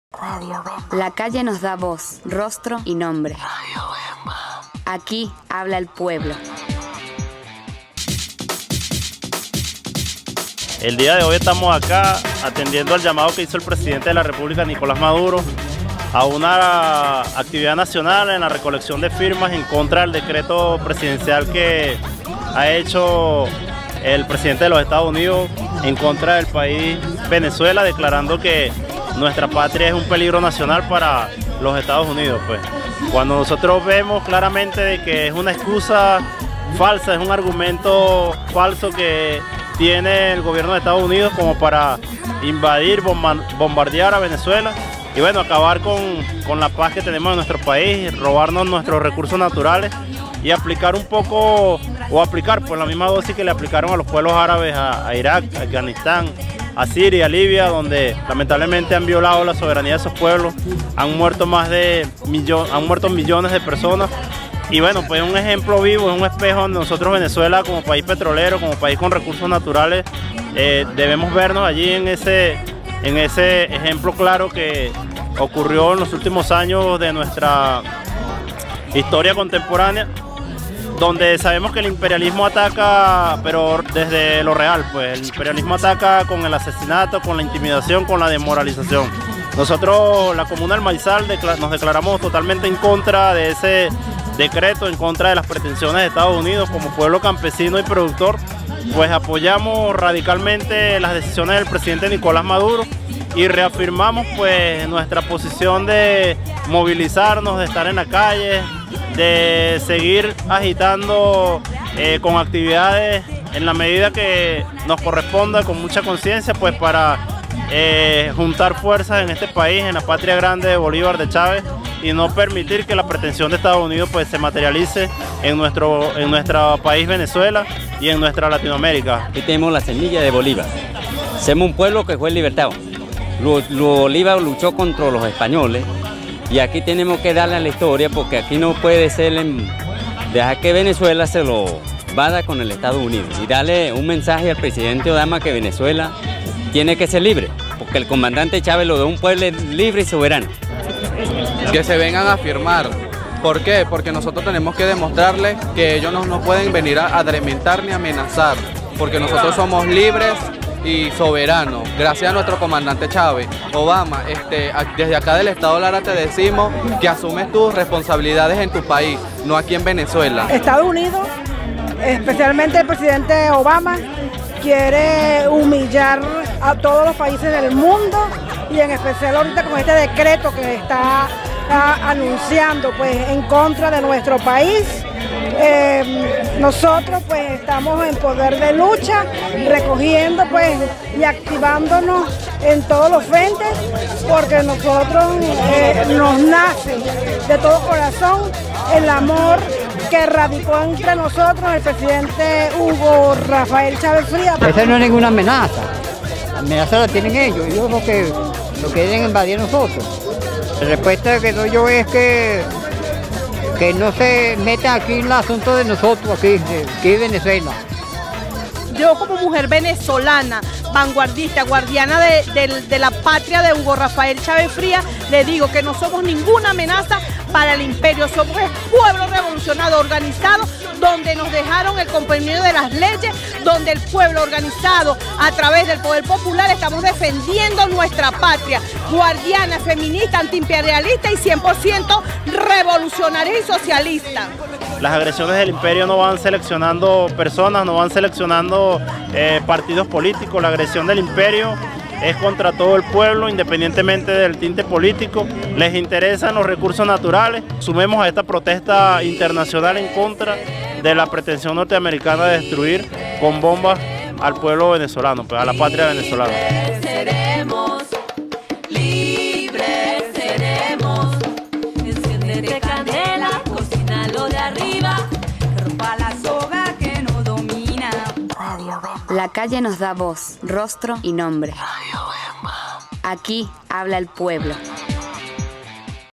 ReporteEnLaCalle ObamaDerogaYa
20 de marzo 2015, barquisimeto, edo Lara

La población de barquisimeto y los municipios cercano, se concentraron en la Plaza Bolivar para firmar y exigirle a Obama a derogar el orden ejecutivo que pinta Venezuela como una amenaza inusual, pero Venezuela ya no se engaña tan fácilmente.  Venezuela no es una amenaza contra de la soberanía de ningún otro país y tampoco se va a dejar su brazo torcer.  Aquí hay un país en revolución y resteado de ser libre.